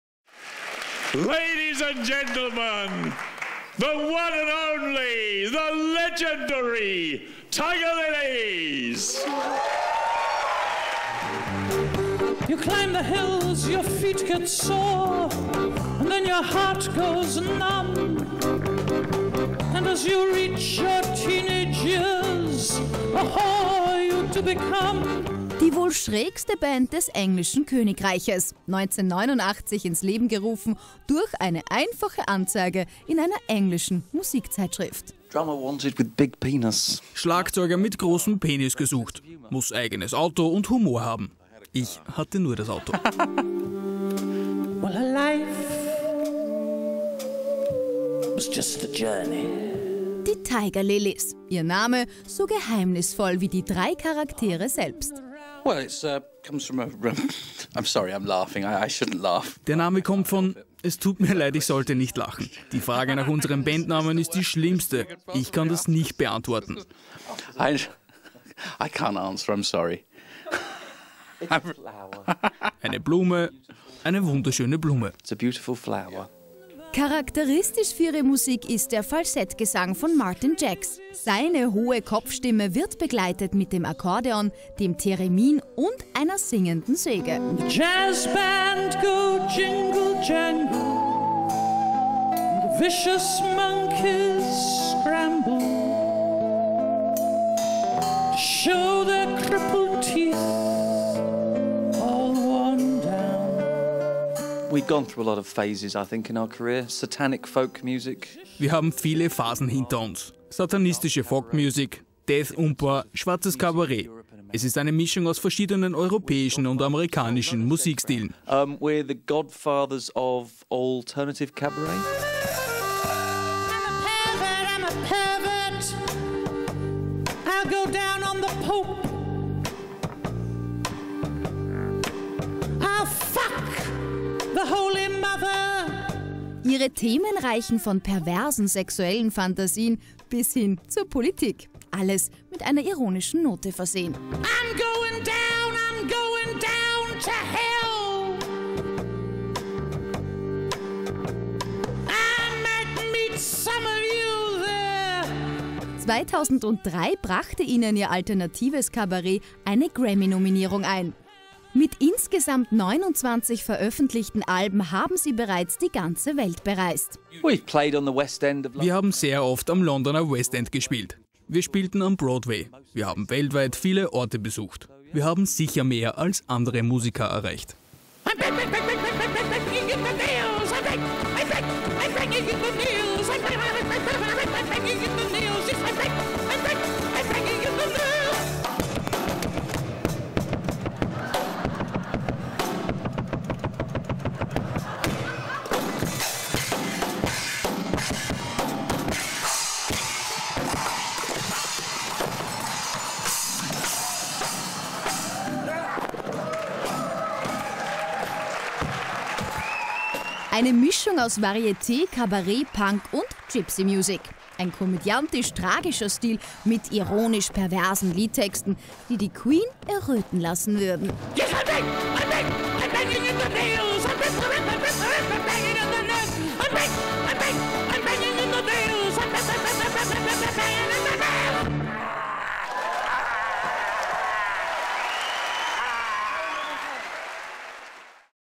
The Tiger Lillies in concert
Gastspiel am Landestheater Linz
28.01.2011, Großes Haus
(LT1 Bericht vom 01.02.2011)